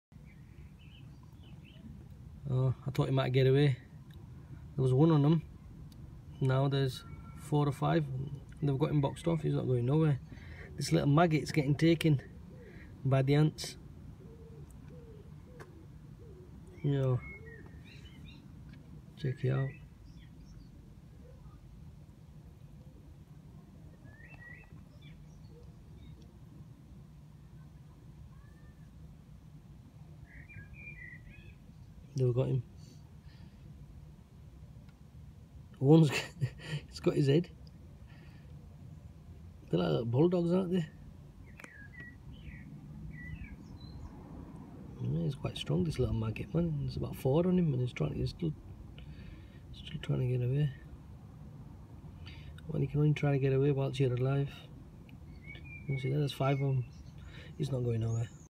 Ants vs maggots
Ants vs maggots , Ants take a maggot alive  the maggot has a lot of strength and keeps fighting for his life